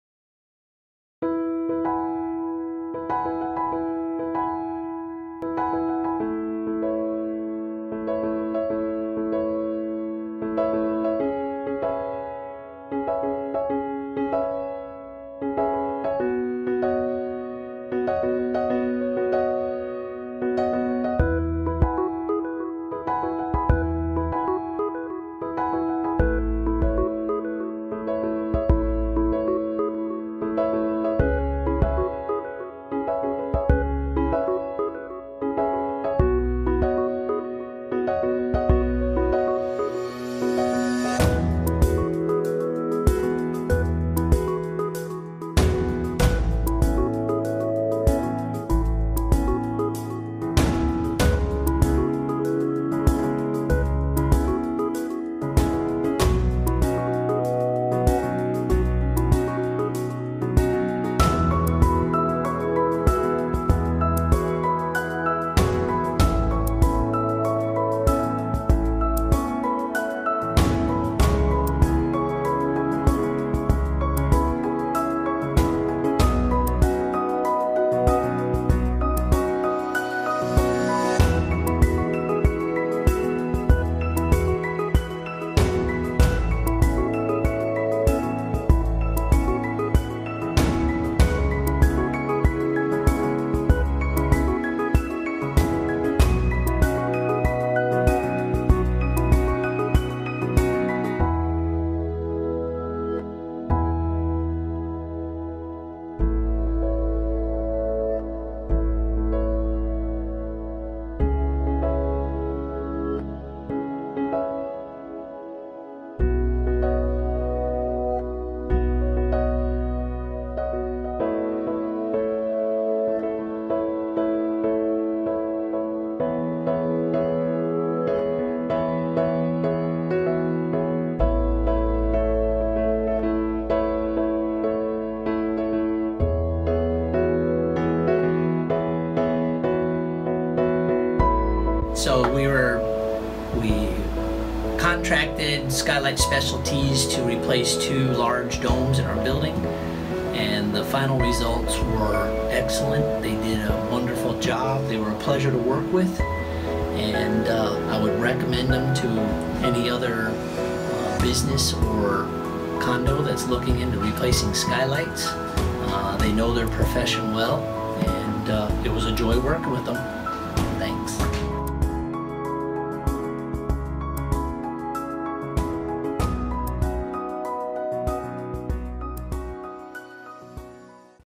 Lausanne Condos Skylight Replacement
Glass dome skylights were installed on the two condo buildings.